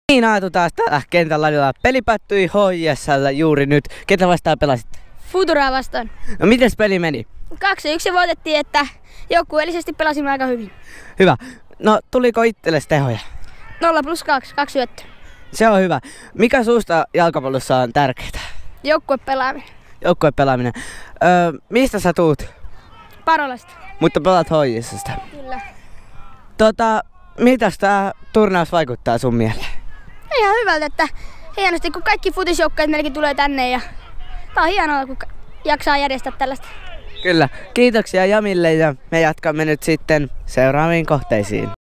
Cumulus turnaus 21.5.2016 Pelaajahaastattelu
Hämeenlinnan Jalkapalloseuran erikoisreportteri Aatu Veikkola vauhdissa!